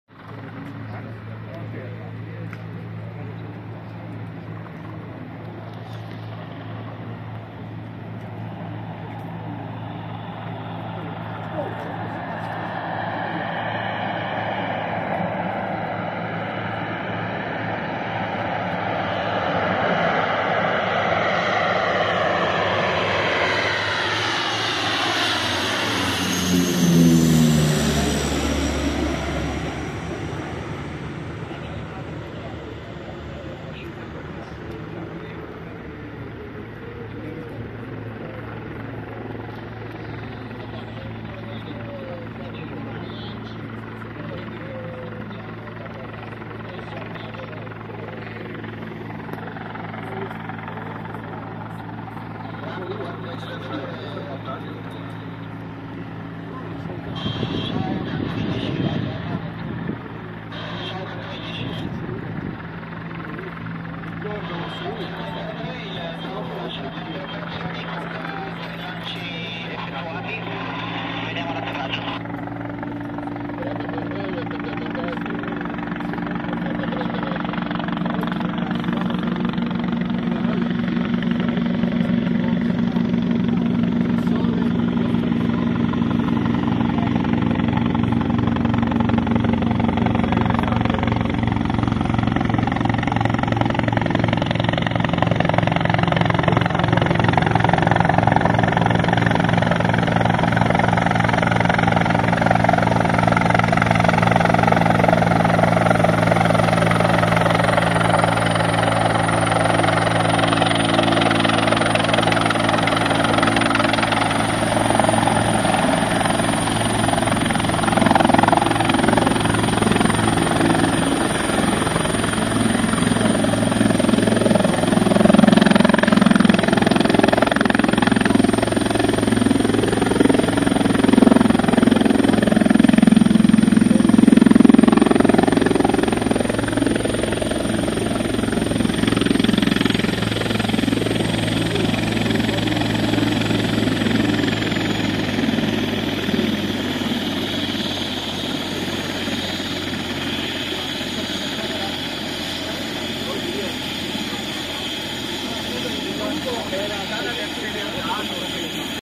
Varallo: centro di formazione per Direttori delle Operazioni di Spegnimento
Varallo: inaugurato il centro di formazione per Direttori delle Operazioni di Spegnimento, della Direzione Regionale VV.F. Piemonte